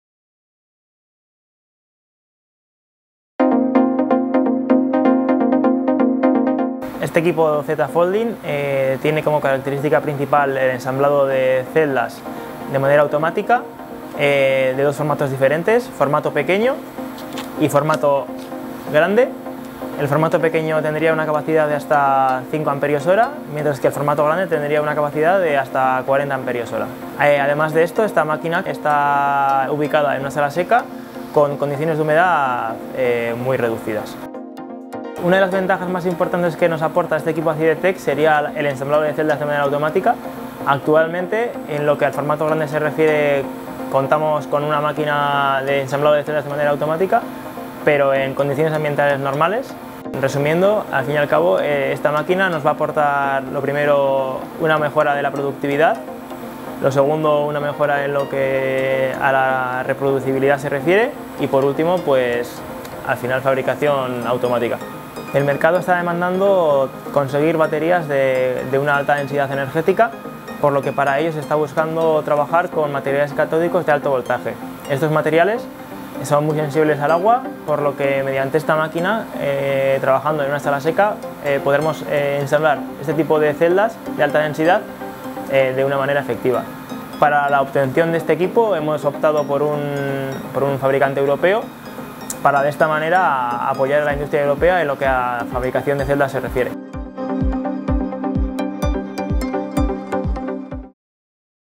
Battery Z-folding machine - CIDETEC Energy Storage
Automatic assembly machine enhances CIDETEC battery cell pilot line facilities.